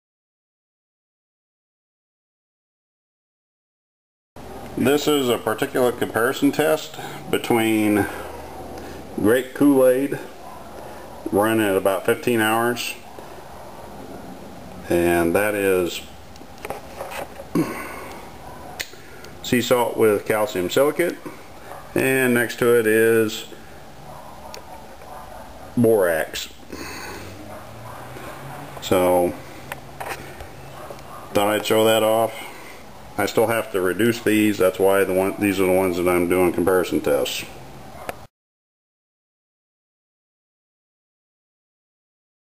#08-077 Grape Kool-Aid Long Term Particulate Comparison Test
I perform a quick comparison between the overnight Grape Kool-Aid electrolysis test and two short duration samples.